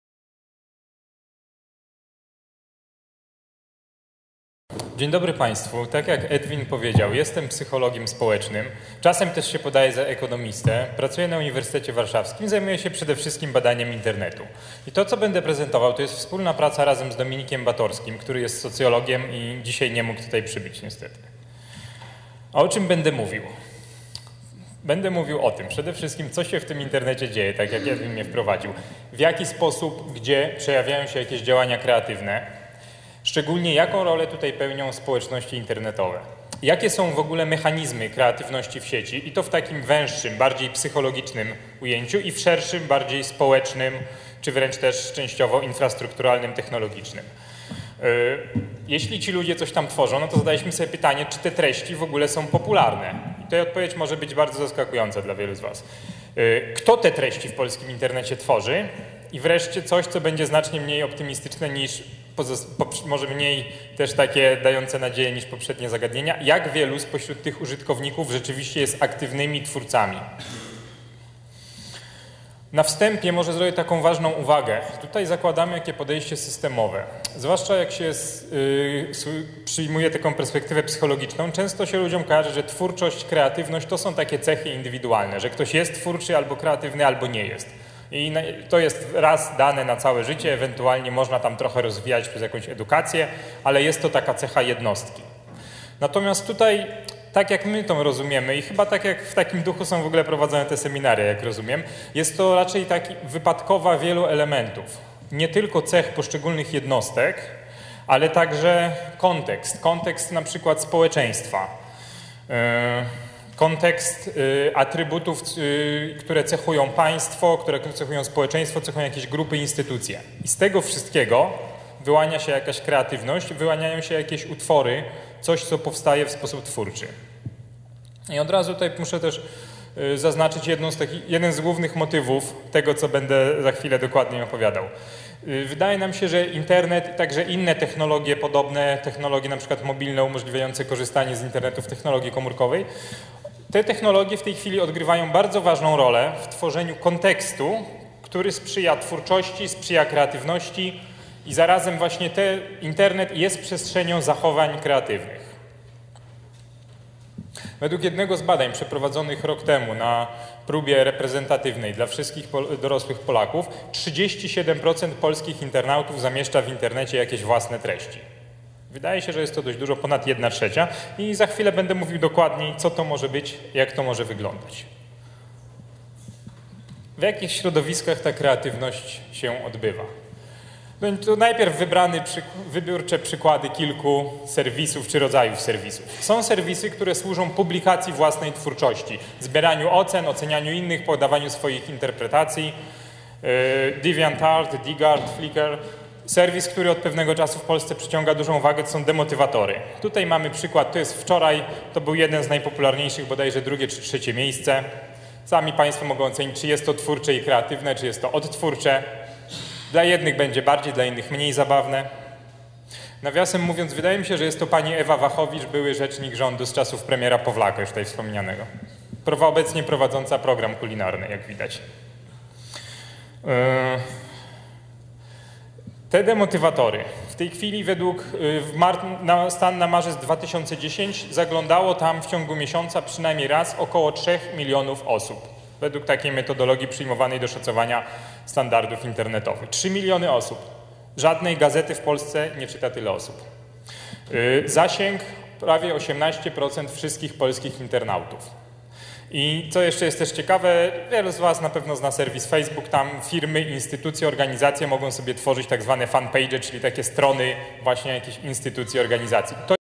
Czy Polacy są kreatywni? [cz.3]
Dr Jan Zając "Potencjał kreatywny cyberprzestrzeni", Collegium Civitas, czerwiec 2010 r.

Czy Polacy są kreatywni? To piąte już seminarium z cyklu „Kultura i rozwój" zorganizowane 9 czerwca 2010 r. przez Ośrodek Badań nad Przyszłością Collegium Civitas i Narodowe Centrum Kultury.

Polska gospodarka jako jedyna z gospodarek europejskich pokonała światowy kryzys „na plusie”, nie przeżywając recesji. Jednocześnie jednak statystyki porównawcze opracowywane przez Komisję Europejską i OECD pokazują, że ta sama zwycięska gospodarka ma duże problemy z innowacyjnością - w tej kategorii plasujemy się na końcu międzynarodowych rankingów.

Nie ma innowacyjności bez przedsiębiorczości i kreatywności. Jeśli chodzi o pierwszy aspekt, Polacy wyróżniają się pozytywnie na tle innych społeczeństw europejskich, już od dziecka marząc o założeniu własnej firmy. Jednocześnie jednak w międzynarodowych pomiarach kreatywności polskie społeczeństwo uzyskuje bardzo niskie wartości indeksów.

Czy Polacy są kreatywni? Jeśli tak, to czy kreatywne jest polskie społeczeństwo? Gdzie szukać zasobów kreatywności zgodnie uznawanych dziś za jeden z kluczowych czynników prorozwojowych? Jak mierzyć potencjał kreatywny?